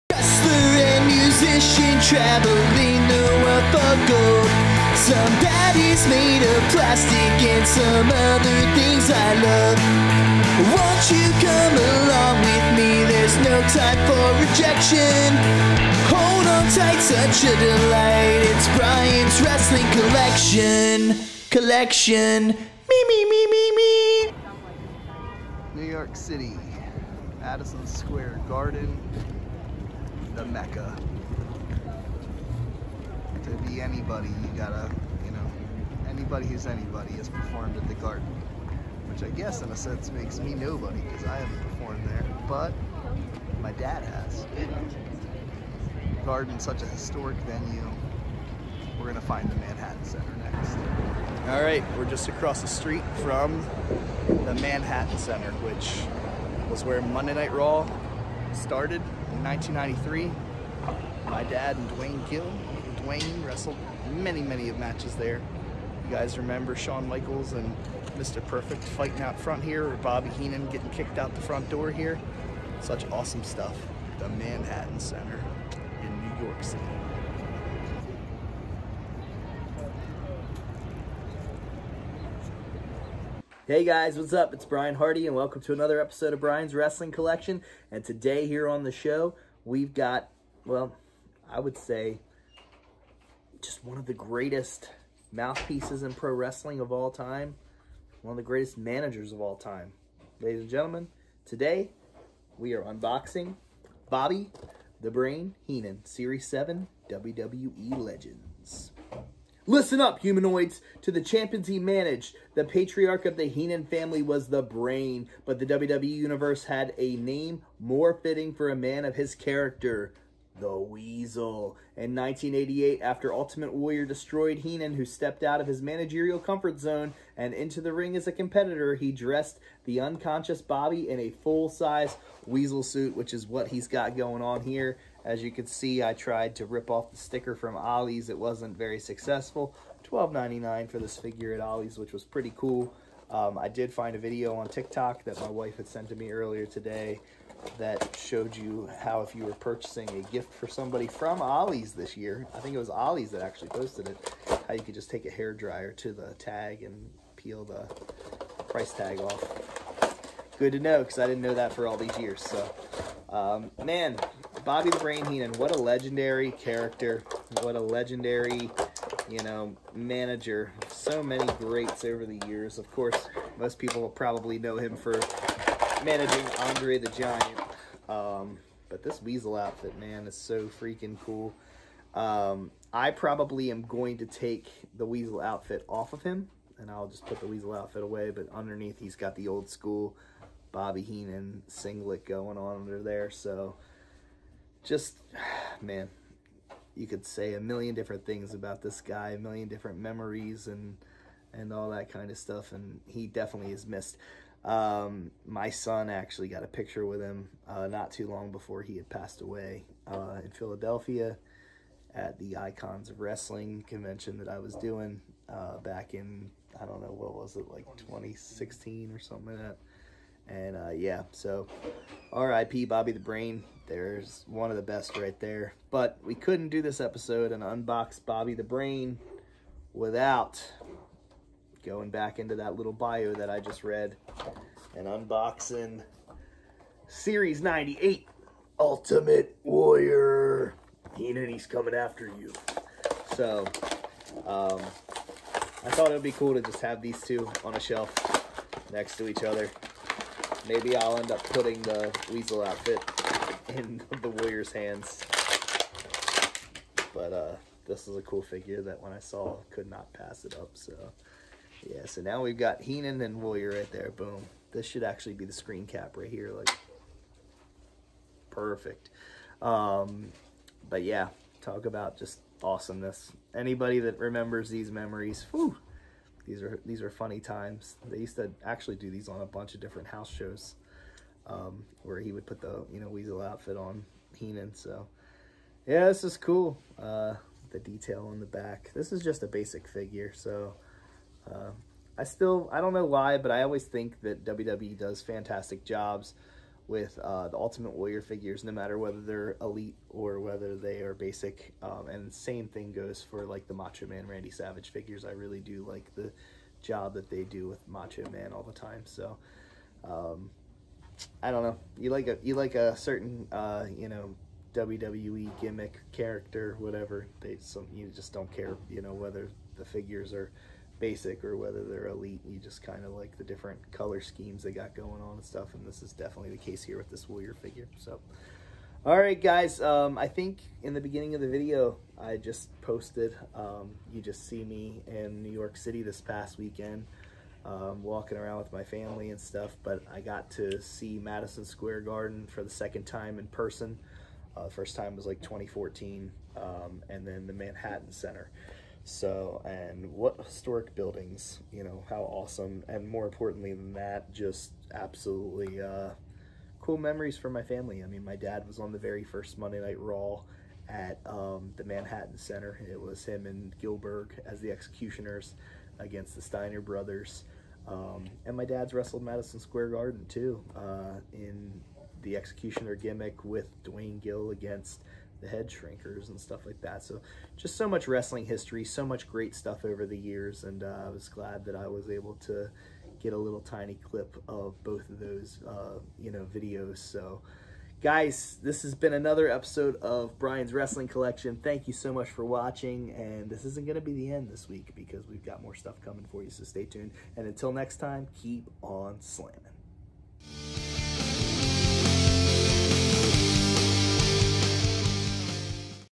Brian's Wrestling Collection (EP119) - Bobby The Brain & Ultimate Warrior Unboxing!
On This Episode of Brian's Wrestling Collection - Brian stops by some very historic venues in NYC! Then Brian Unboxes The Legendary Bobby The Brain Heenan & Ultimate Warrior Figures!